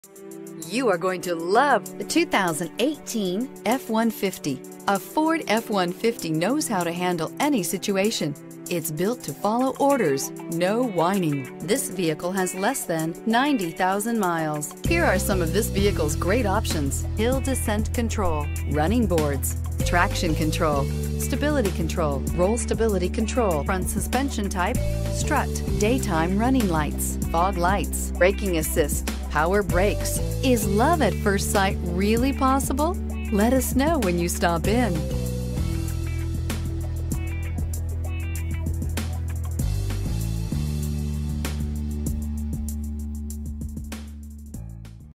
2018 Ford F-150 Houston TX Z645853A
2018 Ford F-150 Raptor

This car is a 2018 Ford F-150 Raptor, with 89,262 miles driven. It has a 3.5 Liter engine and 6 cylinders, Twin Turbo. The exterior color is Oxford White, while the interior color is Black. It has a crash test rating of 5 out of 5 stars, and features such as Bluetooth audio connection, Hill Start Assist, Center Console Trim, Leather Drivetrain, four wheel drive Type: Part Time with On Demand Setting, Bluetooth Phone Connectivity, and Leather-Trimmed Seats. This car is an excellent choice for anyone looking for a reliable motorcar with a great safety rating of 5 out of 5 stars.